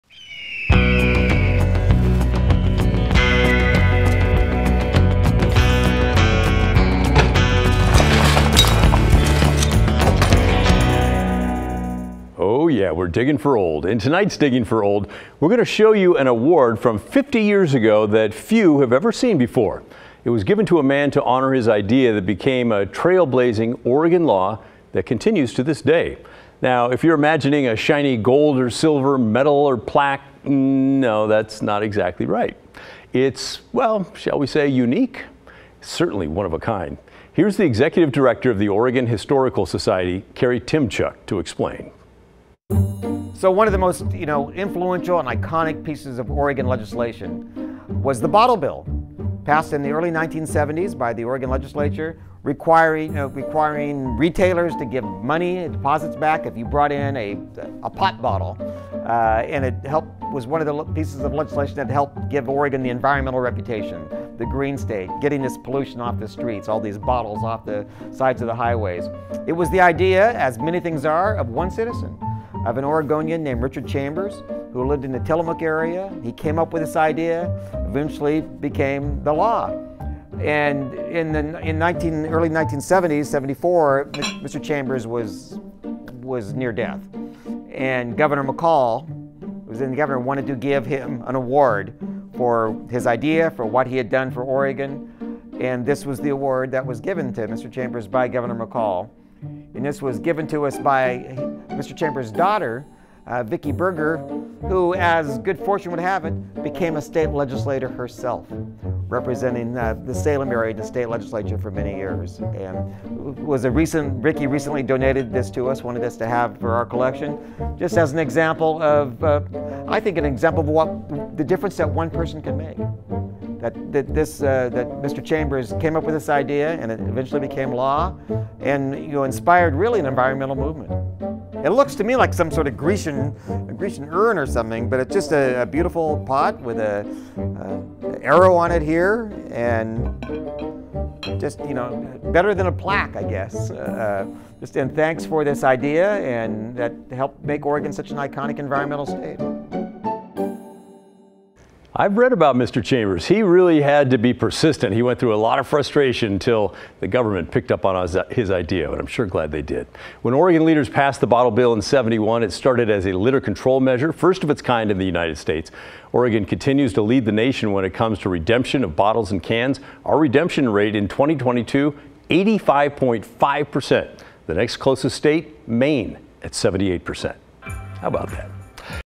Why'd Tom McCall give the Bottle Bill's architect an urn? He earned it | Digging for Old
Richard Chambers came up with the idea that would become Oregon's Bottle Bill. It turned out to be a wildly successful and pioneering anti-littering measure, one that earned accolades from then-Gov. Tom McCall.